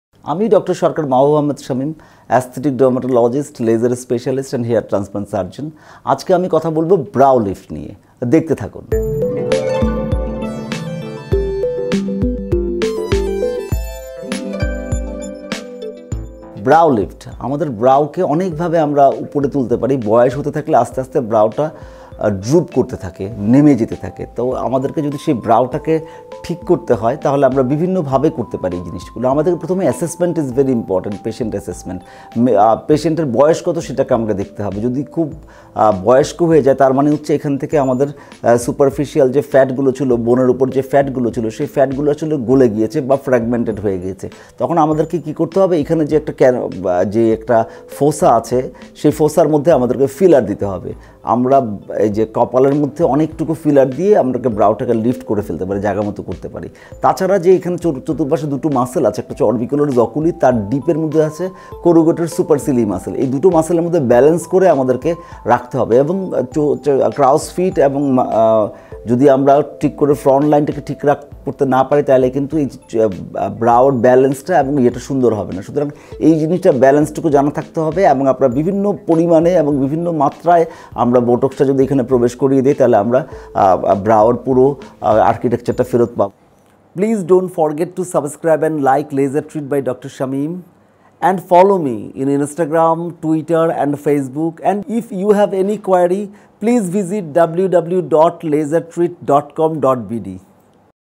বোটক্স ও ফিলার । ডা: সরকার মাহবুব আহমেদ শামীম । লেজারট্রিট
নারী পুরুষ সকলেই চায় তারুণ্য ধরে রাখতে। কিন্তু অনিয়ন্ত্রিত জীবনযাপন, খাদ্যাভাস এবং পরিবেশগত কারণে অনেকেরই অল্প বয়সেই ত্বকে বলিরেখা, রিঙ্কেলস বা ফাইন লাইন দেখা দেয়। আর এই সমস্যার নিরাপদ এবং কার্যকরী সমাধান বোটক্স এবং ফিলার। কিন্তু বোটক্স এবং ফিলার কি? কিভাবে হয় এই চিকিৎসা? কারাই বা করাতে পারেন এটি? এ সম্পর্কে বিস্তারিত জানাচ্ছেন লেজারট্রিট এর অভিজ্ঞ ডার্মাটোলজিস্ট ডা.সরকার মাহবুব আহমেদ শামীম।